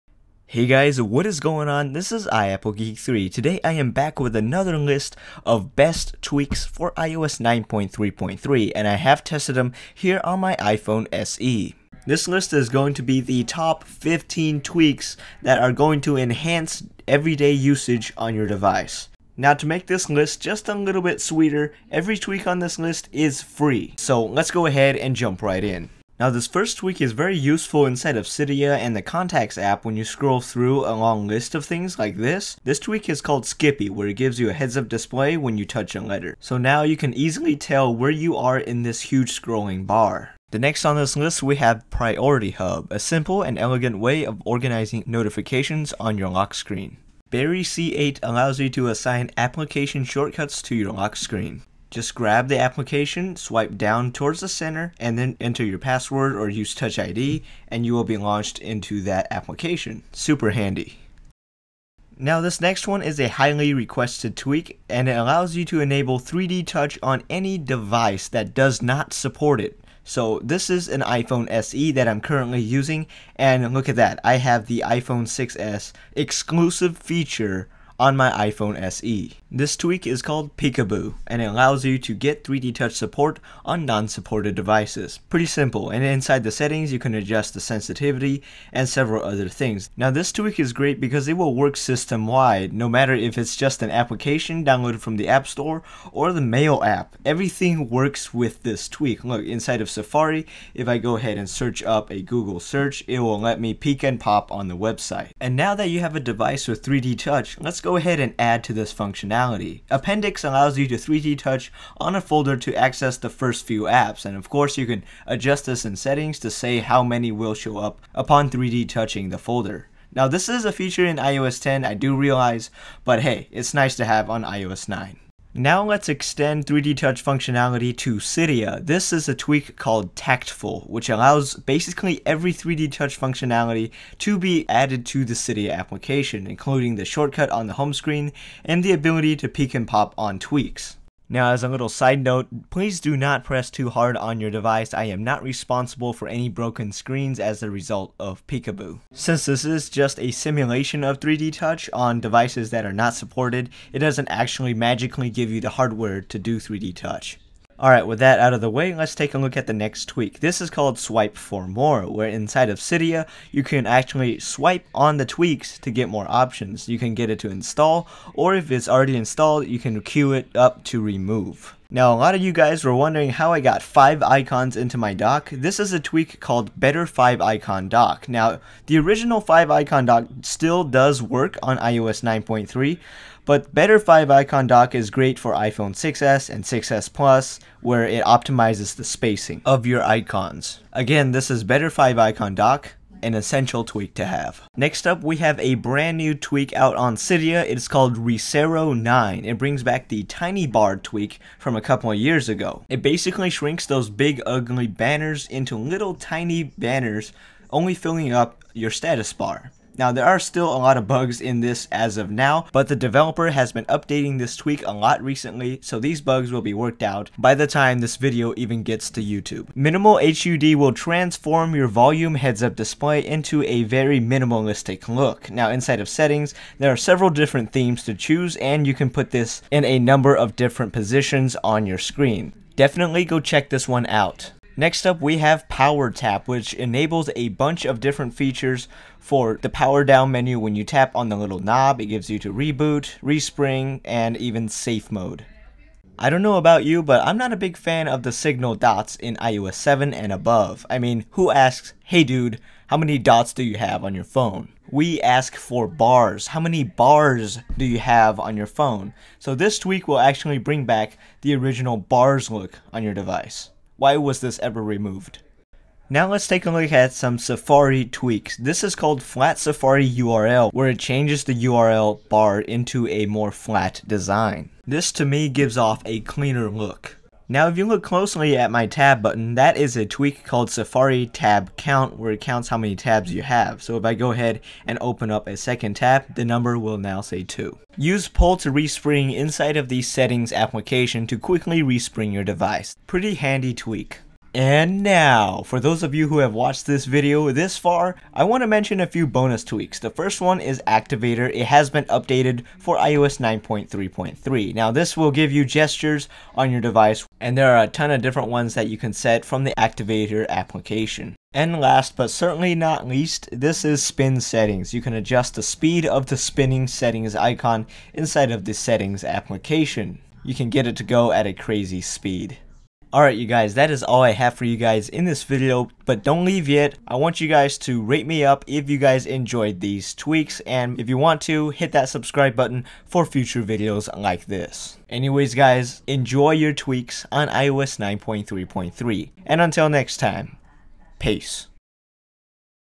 Top 15 Enhancement Tweaks (Free) iOS 9.3.3
Enhance your iPhone, iPad, or iPod touch with these Cydia tweaks! Works with the iOS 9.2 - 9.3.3 jailbreak!

Full List of Tweaks

1. Skippy
2. Priority Hub
3. BerryC8
4. Peek-a-boo
5. Appendix
6. Tactful
7. SwipeForMore
8. betterFiveIconDock
9. Resero 9
10. Minimal HUD
11. PowerTap
12. Bars
13. FlatSafariURL
14. SafariTabCount
15. PullToRespring

Bonus Tweaks

Activator (updated)
SpinSettings